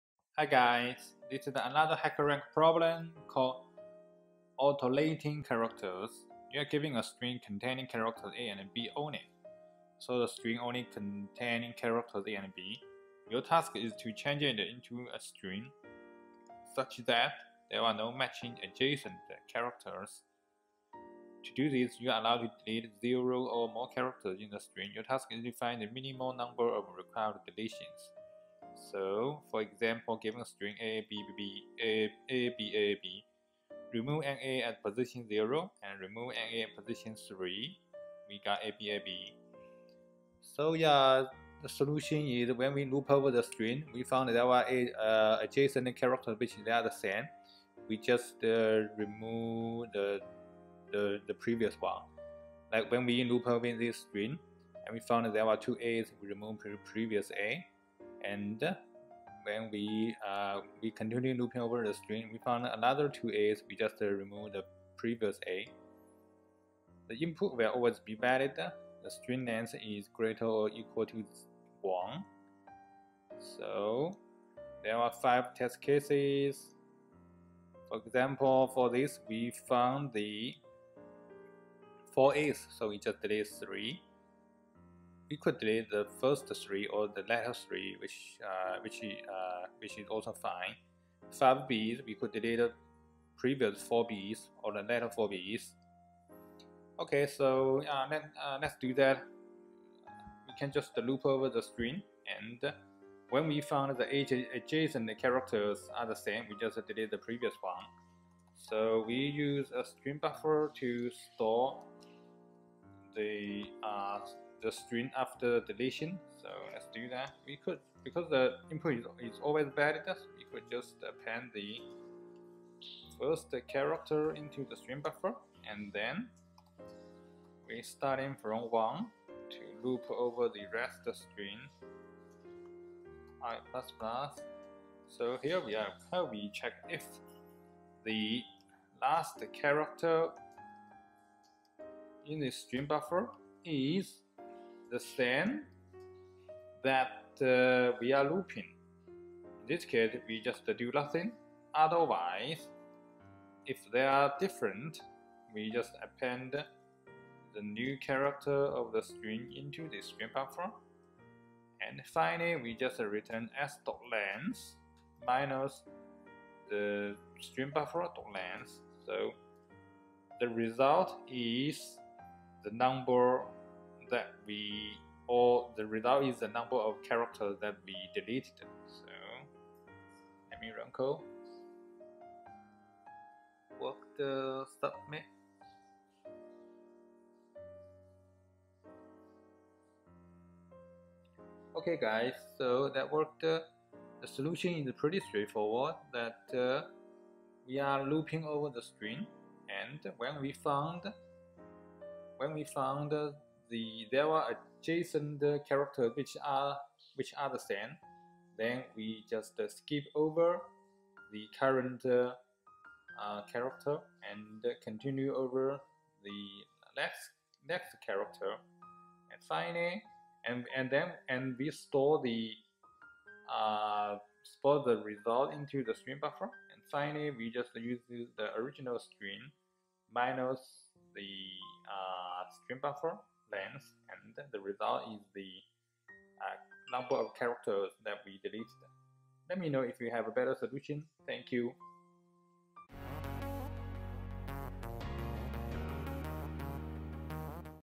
HackerRank - Alternating Characters
With StringBuffer's help to loop over the String to remove the adjacent same character to solve HackerRank Alternating Characters problem.